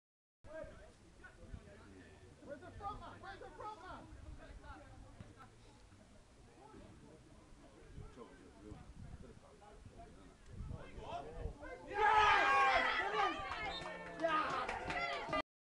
ECPL Premier League Game ,St Stephen 4 v 4 Mount Gould on Saturday 14th Dec 2019
Mount Gould take the lead on  25 minutes.